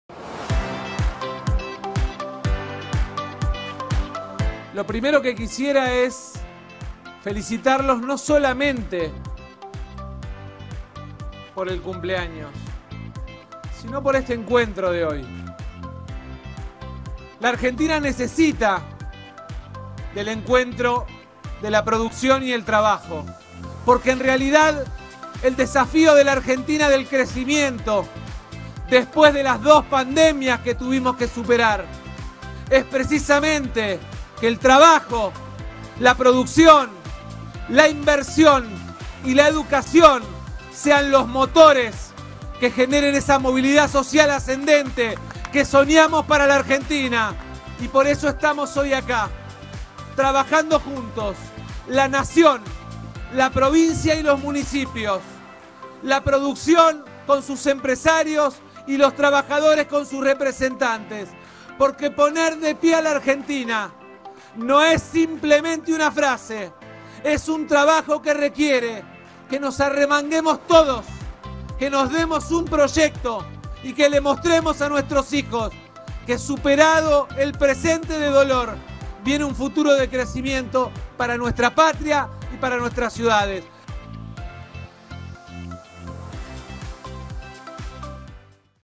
23-2-22 | 121° ANIVERSARIO DE COMODORO RIVADAVIA | ▶️ RESUMEN
Participación en el 121° Aniversario de la ciudad de Comodoro Ribadavia.